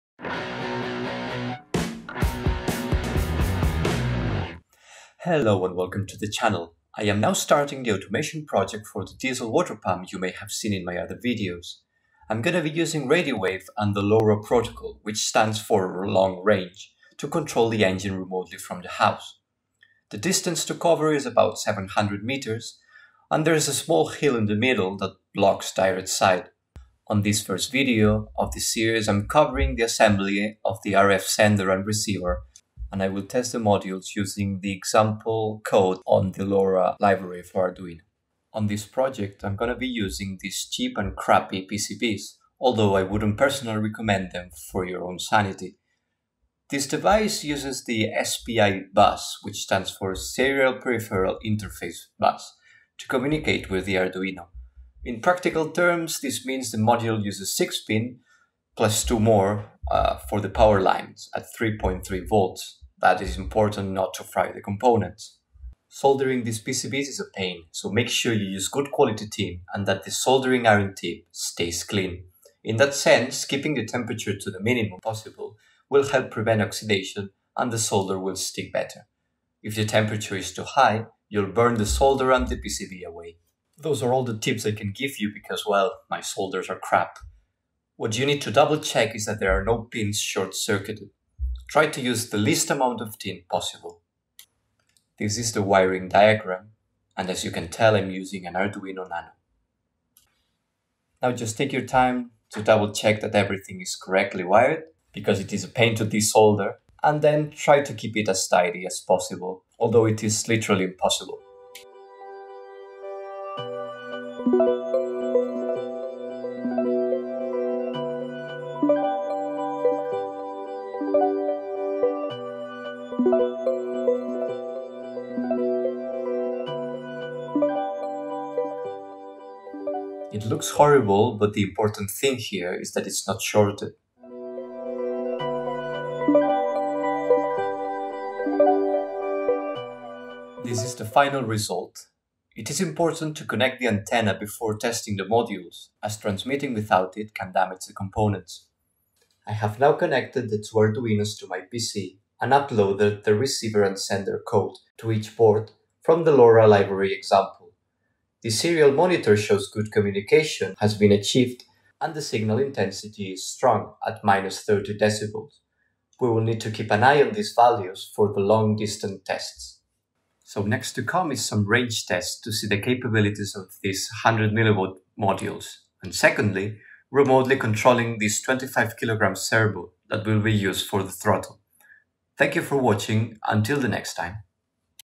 Controlling an engine via LoRa protocol using a Ra-02 RF module & Arduino [Part 1] - The assembly
Hi, this is the beginning of my automation project for the diesel water pump I have restored. The idea is to control the engine from the house (700 meters away), monitoring temperatures and adjusting the throttle for best fuel consumption.
I am using two Arduino nano and two Ra-02 SX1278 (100mW) RF modules that operate at 410-525 Mhz.
*Please check your country radio-frequency regulations before attempting to work with these modules*.

*LINKS to the Restoring an old diesel water pump videos:*